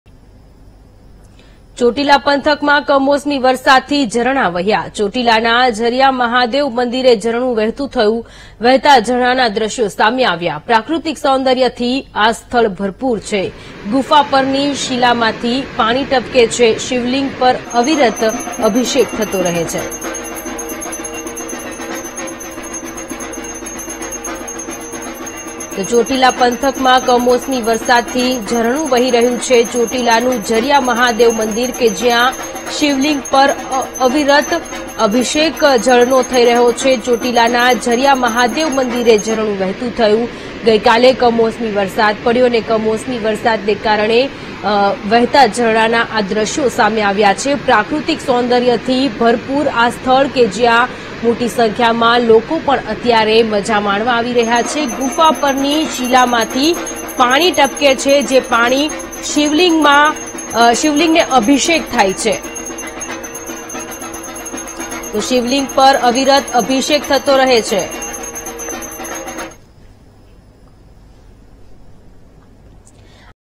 Surendranagar News : ચોટીલા પંથકમાં કમોસમી વરસાદથી ઝરણાં વહ્યાં, વહેતાં ઝરણાંના દ્રશ્યો આવ્યા સામે
Surendranagar News :  ચોટીલાના ઝરીયા મહાદેવ મંદિરે ઝરણું થયું વહેતું, વહેતાં ઝરણાંના દ્રશ્યો આવ્યા સામે I VTV GUJARATI